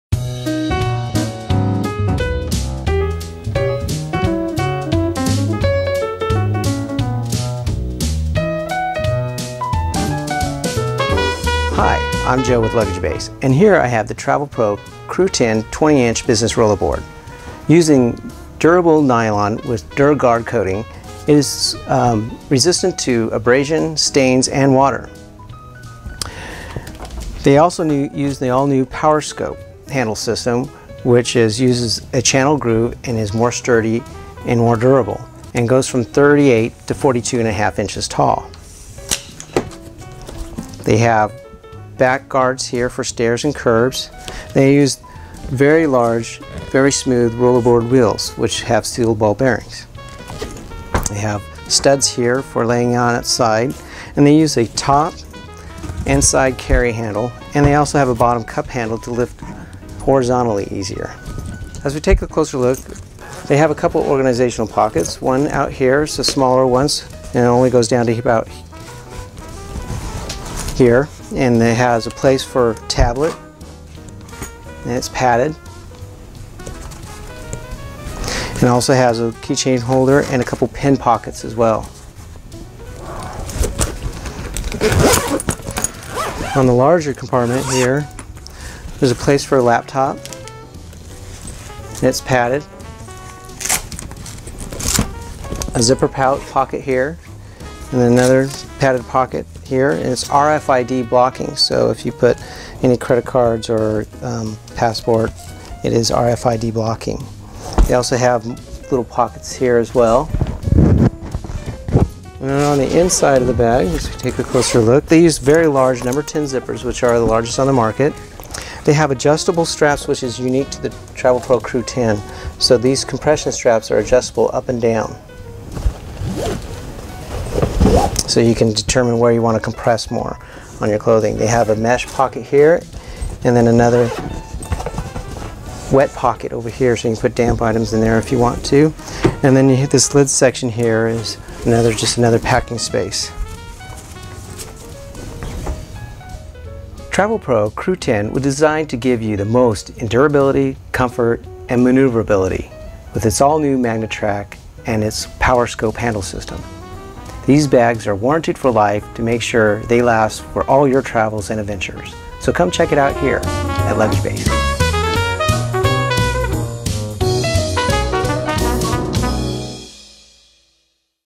Travelpro Crew 10 20 inch Business Plus Rollaboard
Video review of Travelpro Crew 10 20 inch Business Plus Rollaboard by LuggageBase.com shows all the features of the great international carry-on luggage from Travelpro.